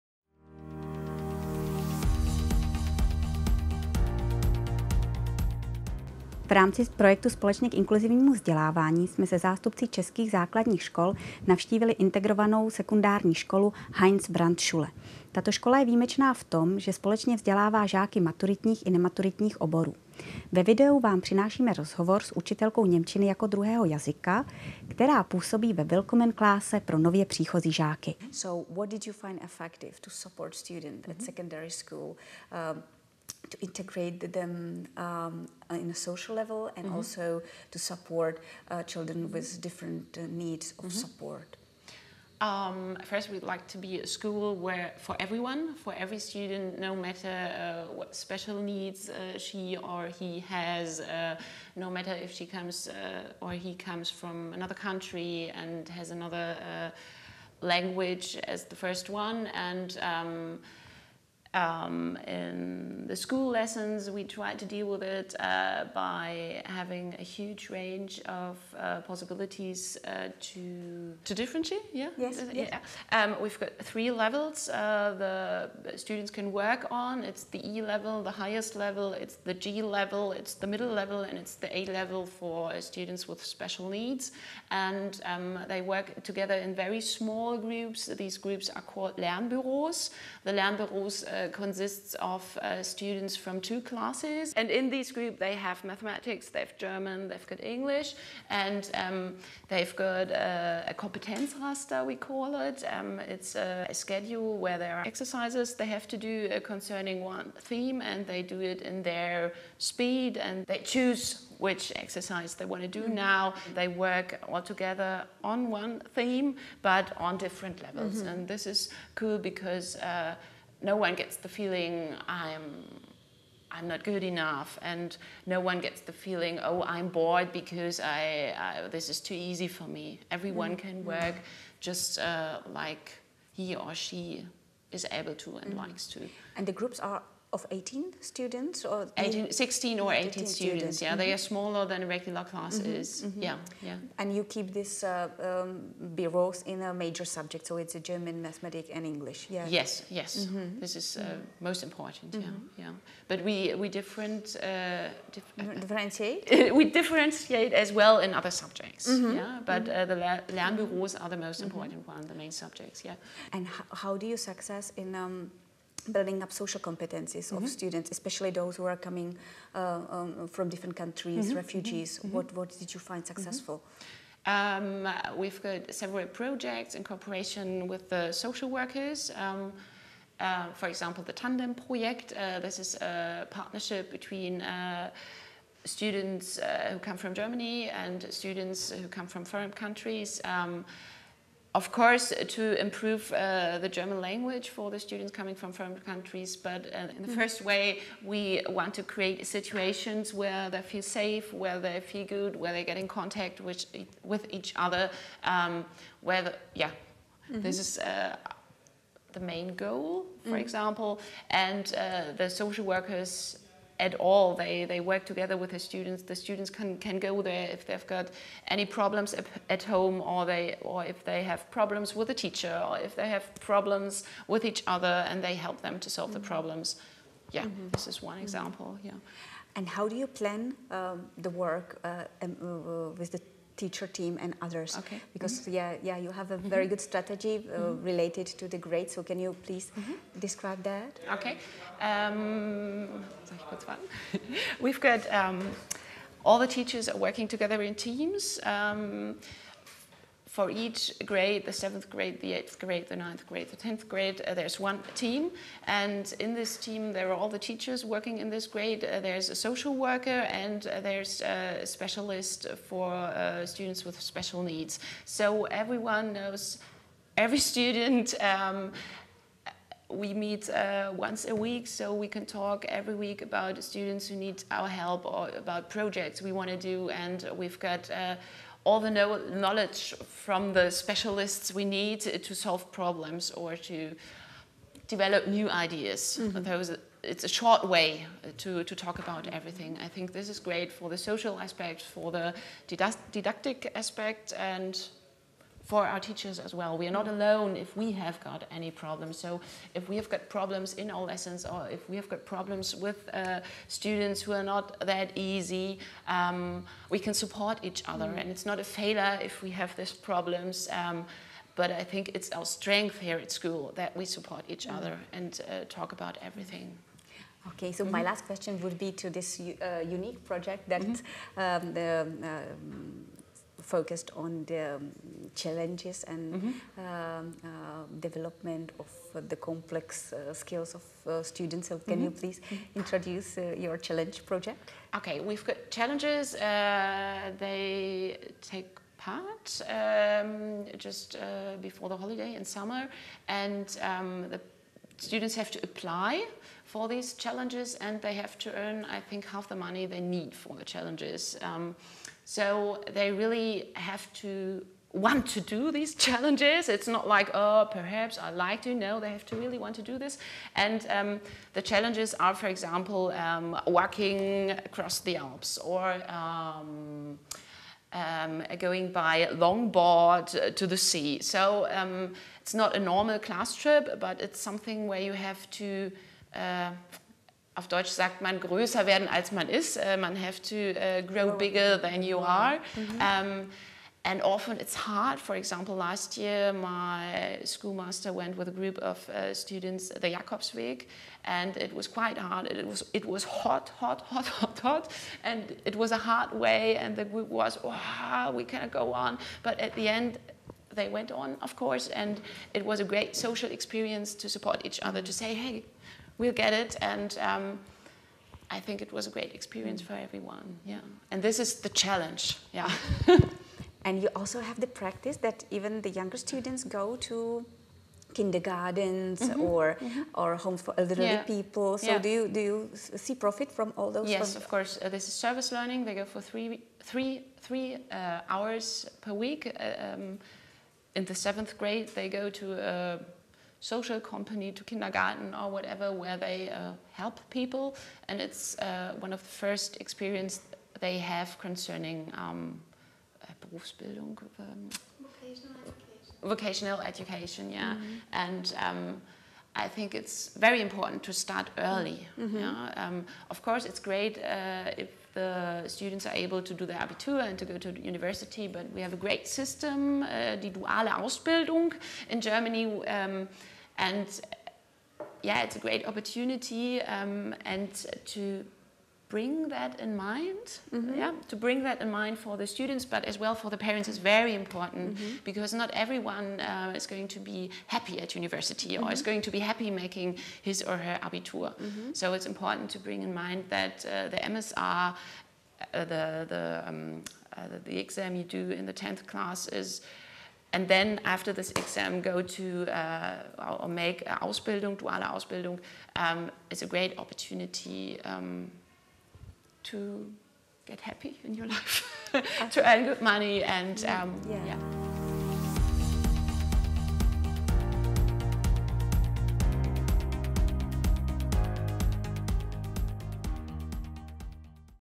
Heinz Brandt Sekundarschule
Zajímá vás, jak může fungovat škola, ve které se ve třídách vzdělávají společně žáci maturitních i nematuritních oborů či žáci s odlišným mateřským jazykem?
Podívejte se na inspirativní rozhovor z návštěvy ČOSIV v integrované sekundární škole Heinz Brandt schule v Berlíně. V rozhovoru s učitelkou němčiny jako druhého jazyka se dozvíte o víceúrovňovém vyučování, efektivním fungování pedagogického kolektivu, spolupráci se sociálními pracovníky či dojemném projektu „Výzvy“. Takto funguje ŠKOLA PRO KAŽDÉHO!